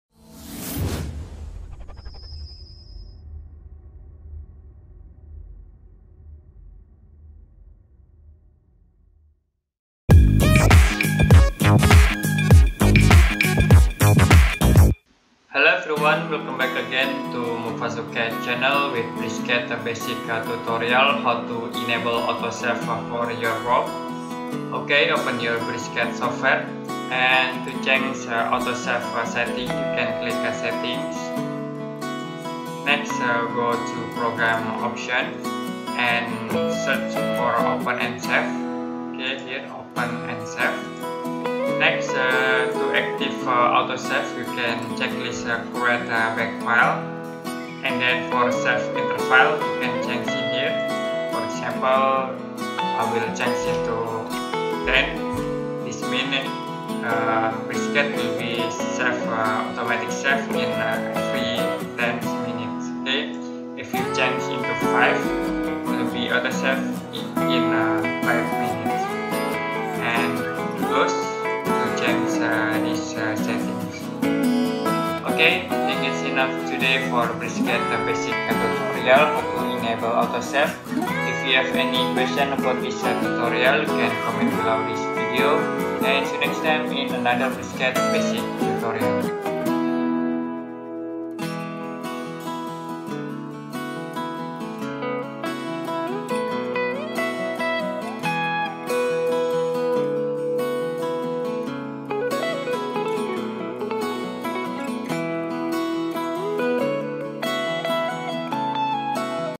Bricscad Enabling Autosave Basic Tutorial For Beginner
Learn briscad basic tutorial for beginner how to enabling autosave and save time interval, check it out!!!